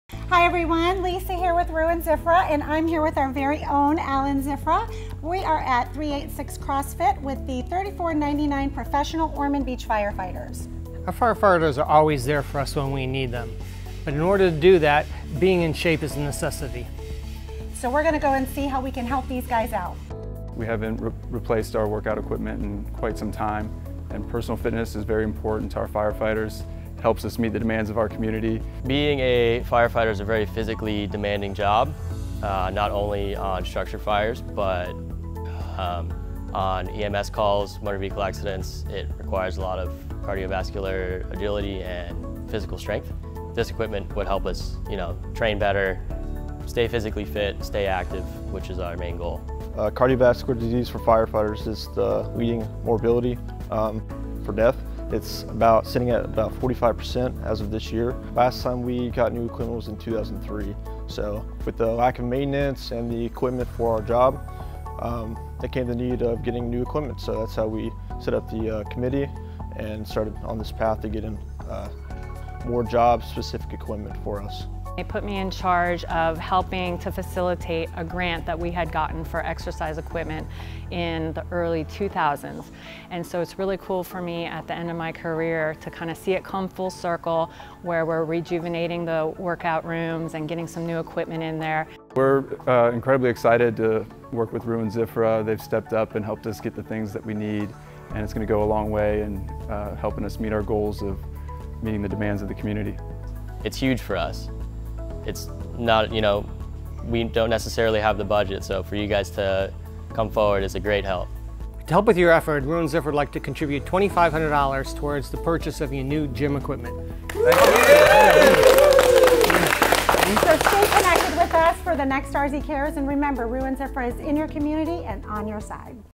RZCares 2022: Local 3499 Ormond Beach Firefighters
In this video, Allan Ziffra is on hand to donate to the Ormond Beach Firefighters Charities to help them buy new gym equipment. Our team is passionate about giving back and supporting those who dedicate their lives to helping others. We hope you'll join us in thanking these brave men and women for their service.

In the Community, On Your Side

Rue & Ziffra
1-800-JUSTICE
In Your Community. On Your Side.